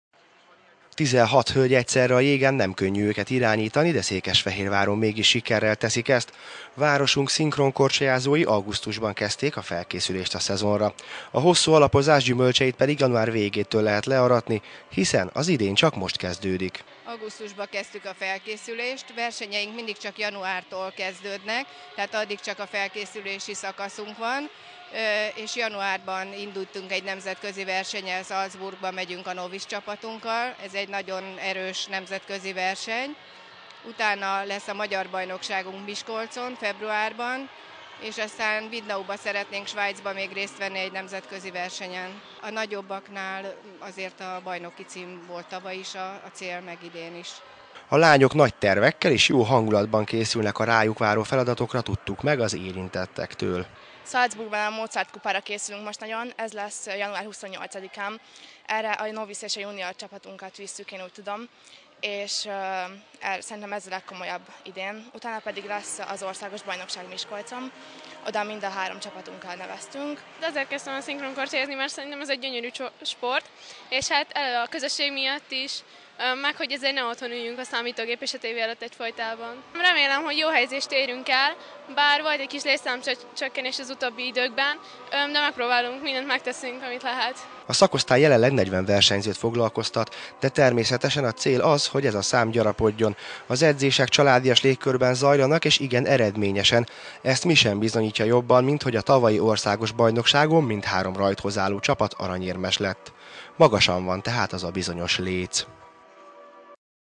Címvédőként készülnek
A szinkronkorcsolyázók is elkezdik lassan a versenyszezont. Mindhárom fehérvári csapat címvédőként készül a februári OB-ra.